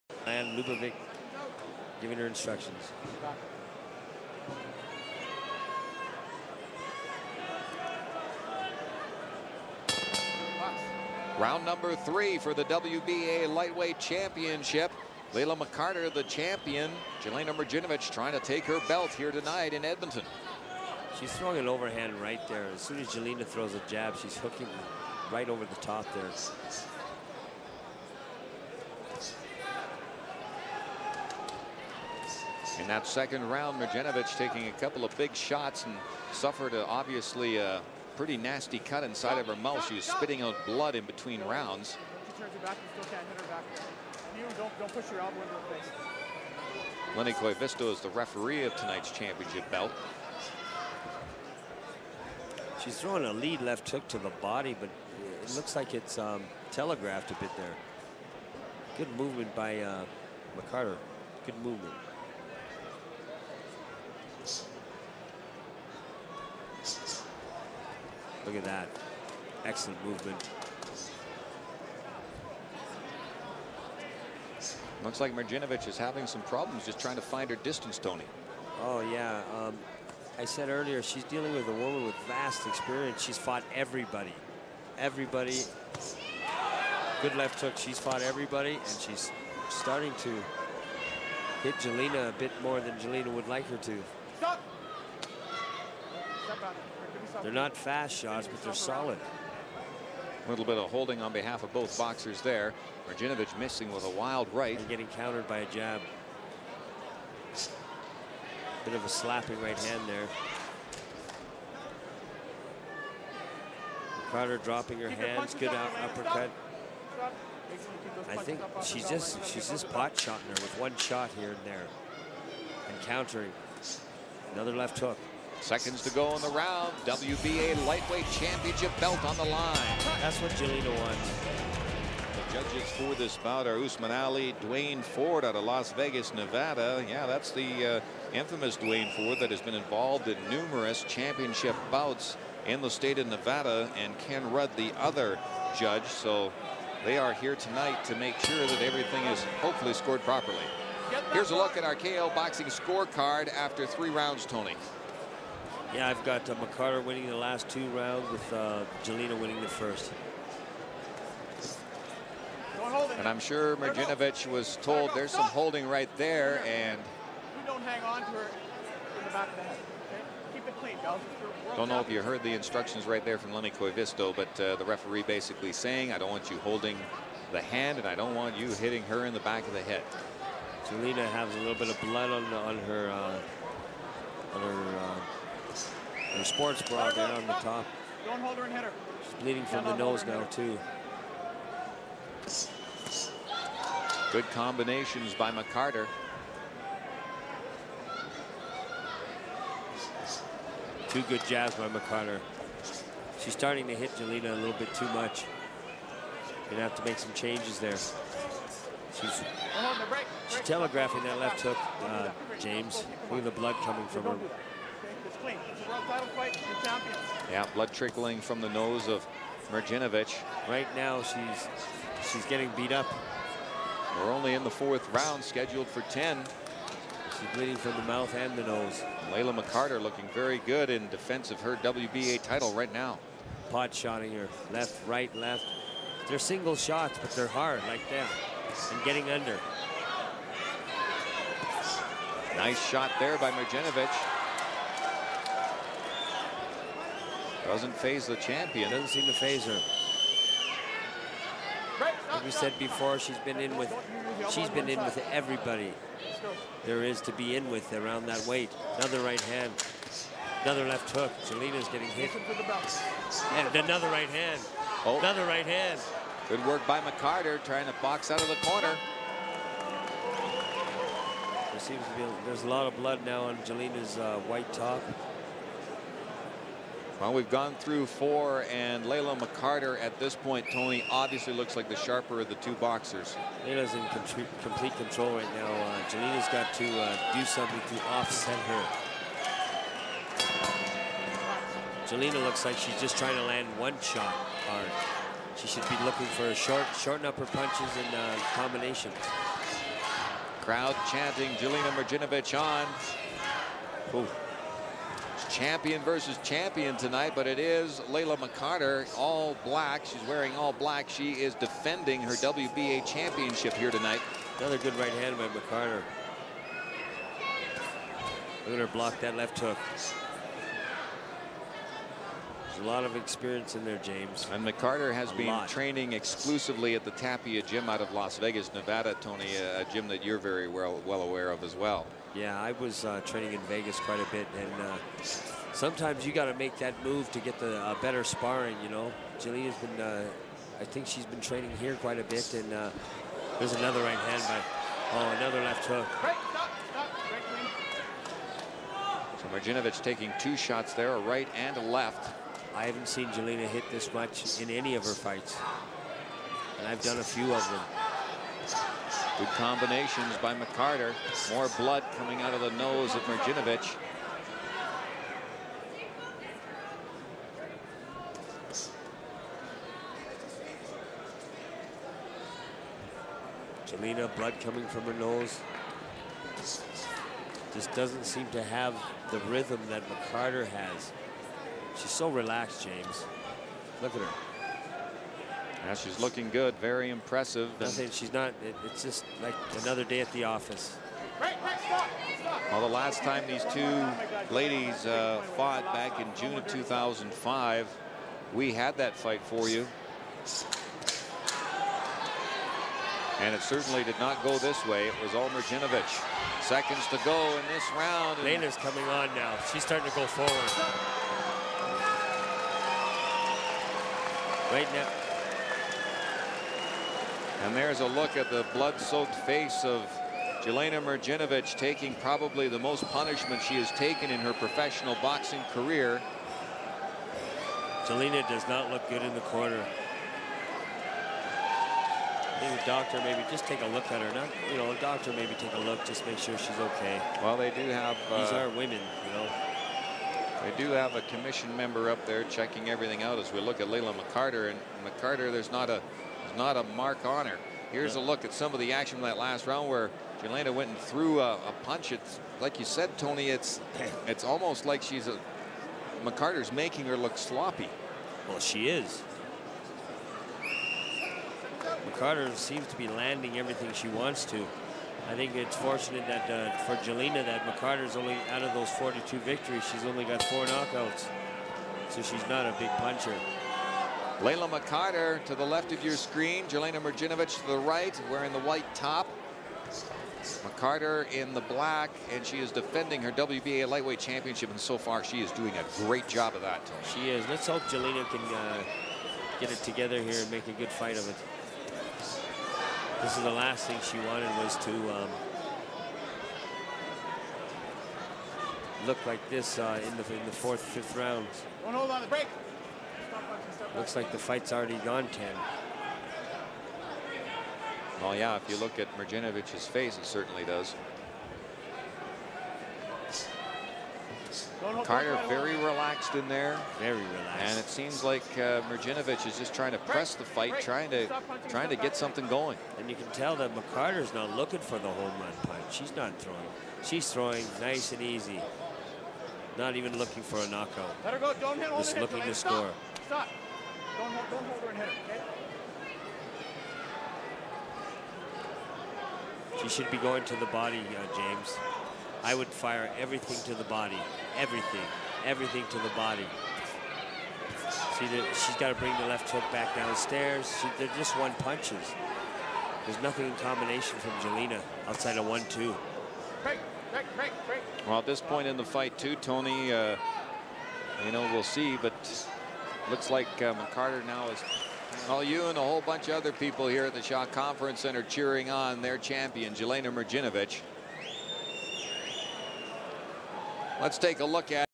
Layla McCarter vs Jelena Mrdjenovich III - part 2 of 3
"CHAMPION VS CHAMPION"
"Amazing" Layla McCarter defending the WBA Lightweight World Title against WBC Lightweight World Champion Jelena Mrdjenovich
March 28,2008 - Shaw Conference Center - Edmonton/Alberta, Canada